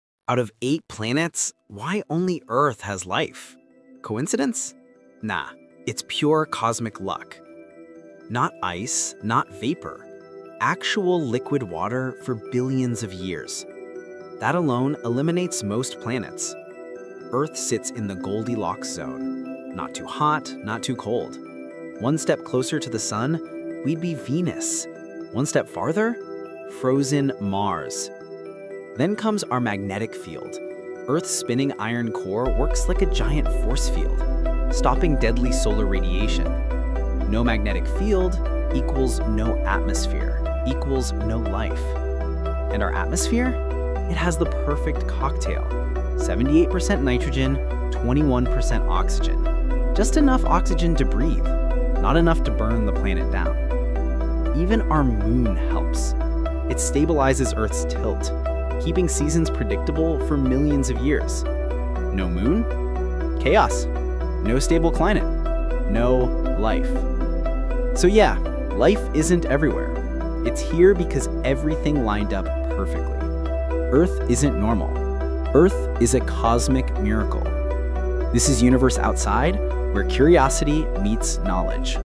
Why Earth is the ONLY planet with life?| Universe Outside
Earth isn’t special by chance — it’s a cosmic miracle. 🌍✨
Liquid water, perfect distance, magnetic shield, stable atmosphere…
Everything lined up so perfectly that life became possible.

Why no other planet has this?
Find out in 60 seconds.

Welcome to Universe Outside — Where curiosity meets knowledge.